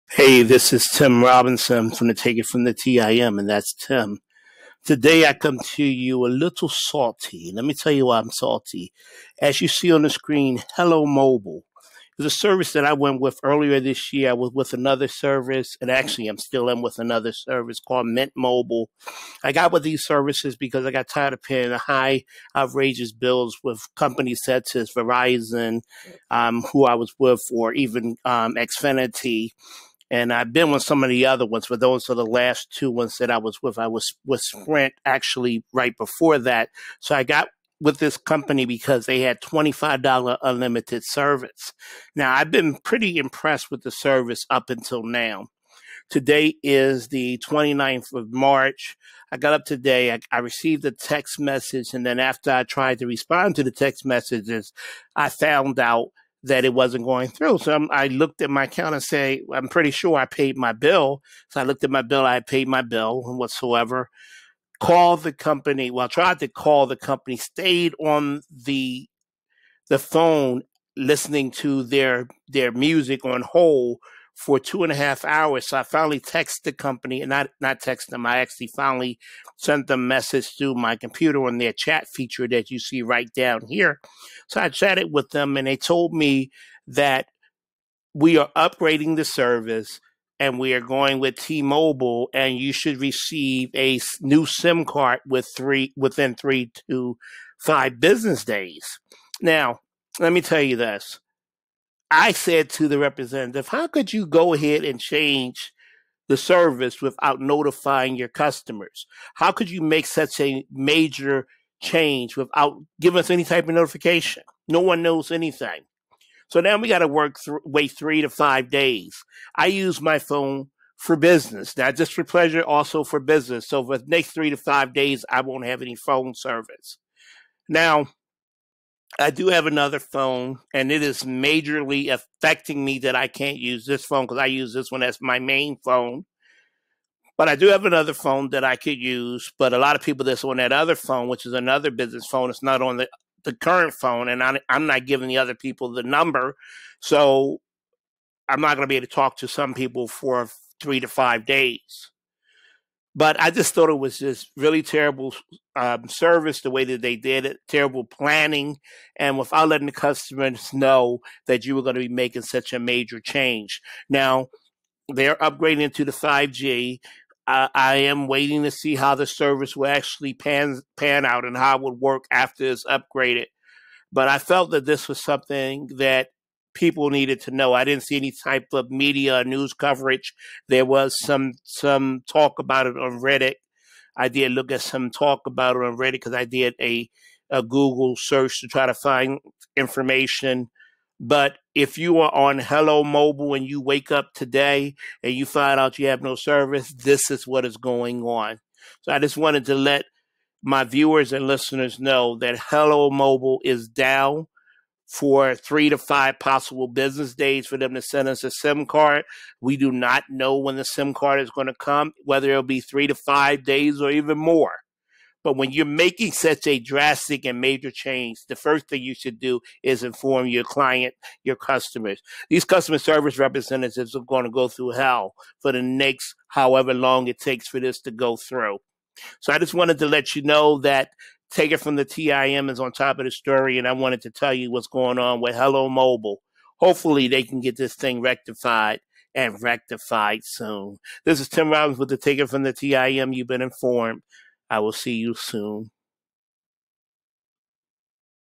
Hello Mobile Shuts down service for Service Upgrade
Many are waking up and finding they have no service with Hello Mobile. I found out when trying to make a phone call.  Called the company and waited on hold for 2.5 hrs to never receive an answer to my call.  Used the chat feature on the internet to find they upgrade to Tmobile 5G service and disabled the current sim cards, without giving a notice to its customer base. I have been impressed with their service but this is a major disappointment!!